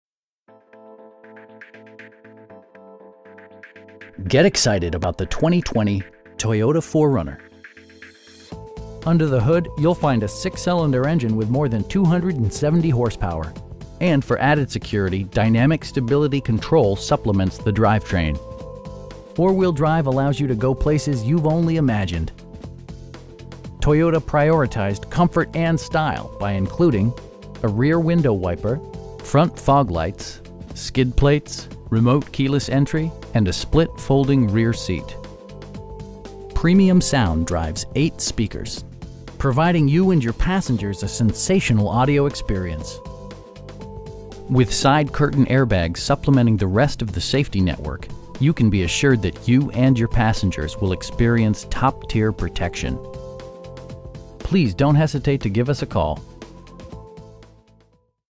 2020 Toyota 4Runner TRD Off Road in Merriam, KS 66203
9505 W. 67th Street

Get excited about the 2020 Toyota 4Runner. Under the hood you'll find a 6 cylinder engine with more than 270 horsepower, and for added security, dynamic Stability Control supplements the drivetrain. Four wheel drive allows you to go places you've only imagined. Toyota prioritized comfort and style by including: a rear window wiper, front fog lights, tilt steering wheel, skid plates, power windows, remote keyless entry, and a split folding rear seat. Premium sound drives 8 speakers, providing you and your passengers a sensational audio experience. With side curtain airbags supplementing the rest of the safety network, you can be assured that you and your passengers will experience top-tier protection. We pride ourselves in consistently exceeding our customer's expectations. Please don't hesitate to give us a call.